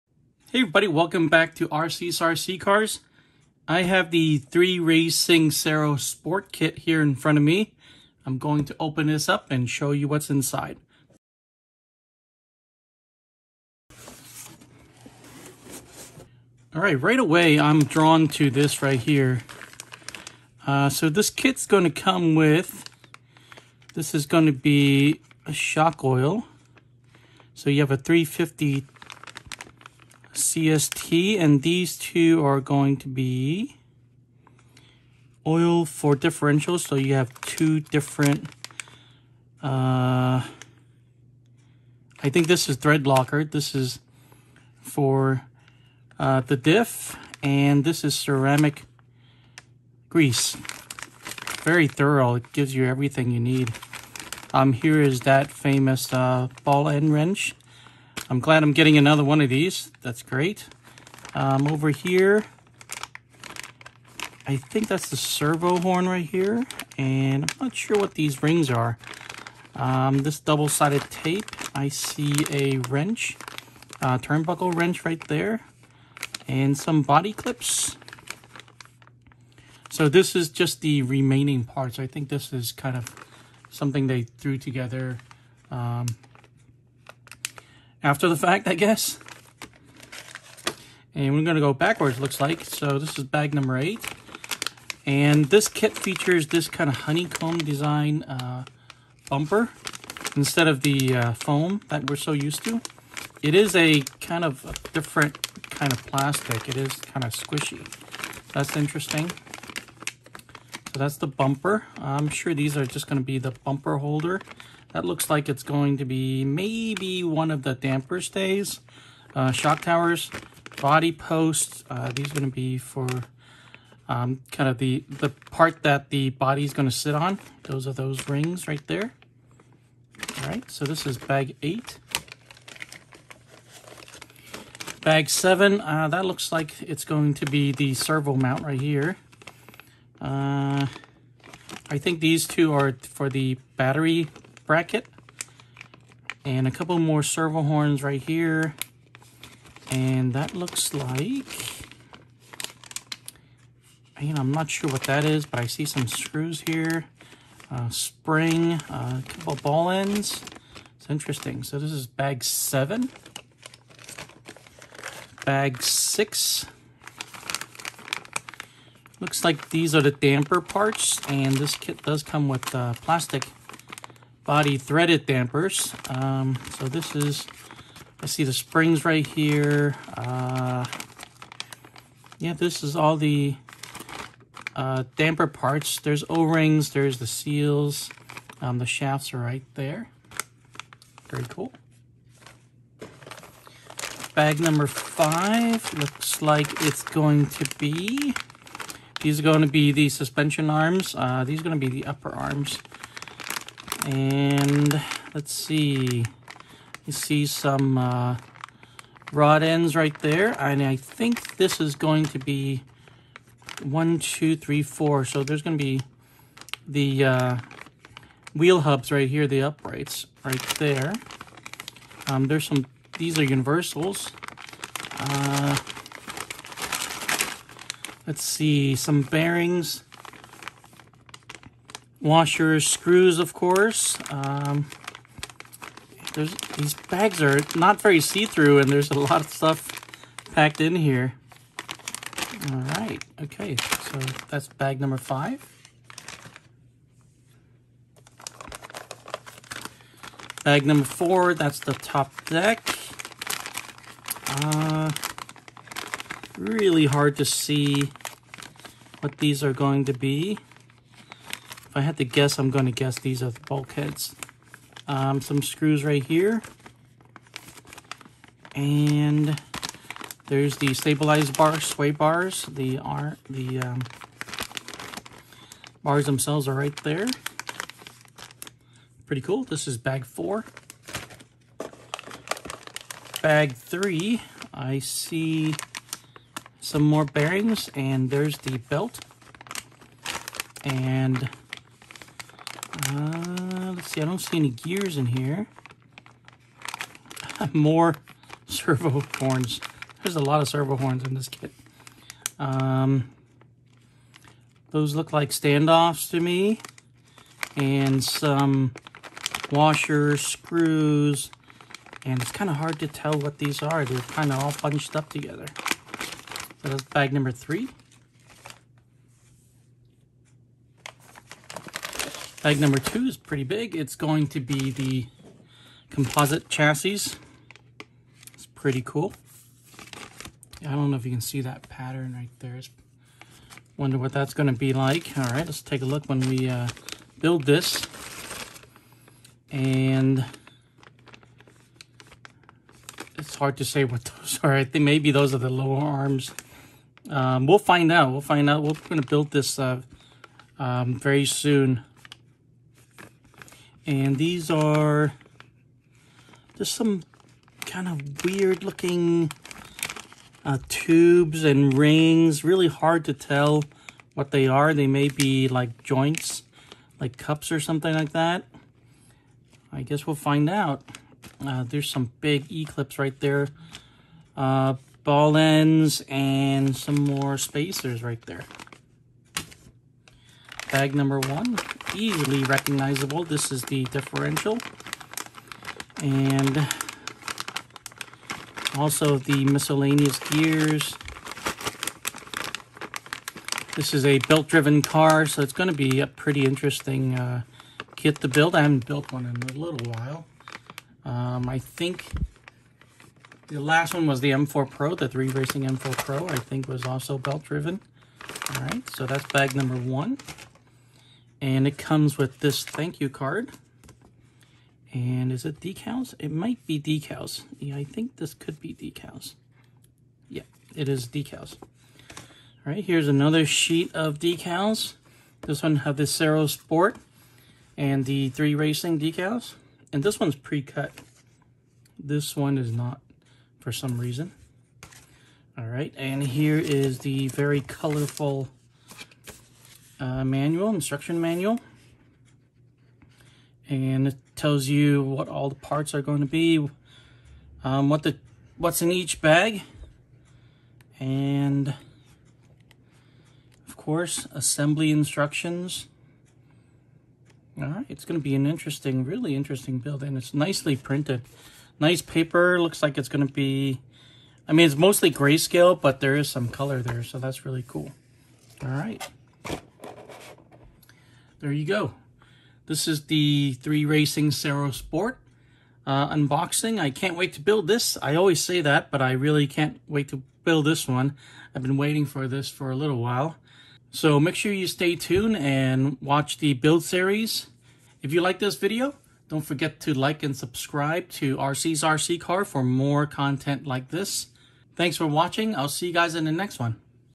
3Racing Cero Sport Unboxing
In this video, I unbox a 3Racing Sakura Cero Sport 1/10 Touring Car Chassis Kit. I'll show you everything comes in the box. I will look at every parts tree and parts bag.

This is the newest 3Racing touring car chassis. It is the Sport version of the acclaimed Sakura Cero Ultra. The Sakura Cero Sport brings an affordable racing chassis to beginner and seasoned racers alike.

0:00 Intro
0:17 Remaining Parts Bag
1:31 Bag 8
2:13 Bag 7
2:43 Bag 6
3:13 Bag 5
4:08 Bag 4
4:42 Bag 3
5:22 Bag 2
6:20 Bag 1
6:52 Decals
7:17 Instruction Manual
7:52 Outro

3Racing Cero Sport Features

1. The first sport touring car platform to utilize pivot ball suspension arms.
2. All suspension geometry carried forward from the full option pro chassis, Cero Ultra.
3. Active Rear Suspension (ARS) allows infinite adjustment to suit track conditions.
4. Lower suspension arms in composite material, one-piece, lightweight, durable and provide additional flex.
5. Mid-motor mount for optimal balance and consistency.
6. Motor mount provides a wide range of gear ratios, to suit blinky spec racing across all winds.
7. One-piece top deck.
8. Quick change spur gear mount.
 9. Molded TPU front bumper.
 10. Steering knuckles are common on all 4 corners, reducing the number of spare parts required.
 11. One-piece bulkhead covers provide consistency and reduces the chance of chassis tweak.
 12. Spring-loaded quick-change battery retainer system.